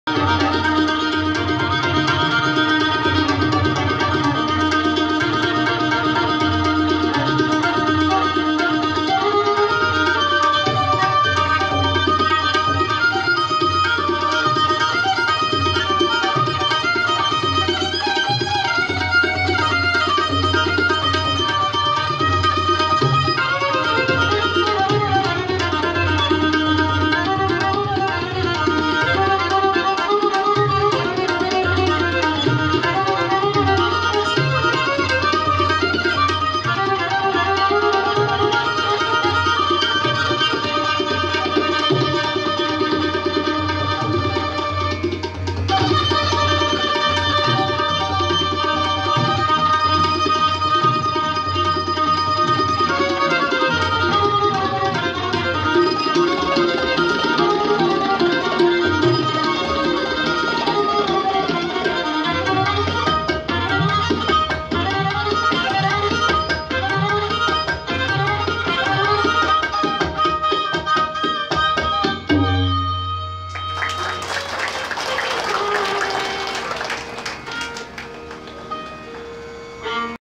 Raag Jayjawanti. Violinist Minoti Khaund
Sangeet Natak Academy concert, Guwahati Assam. May 2016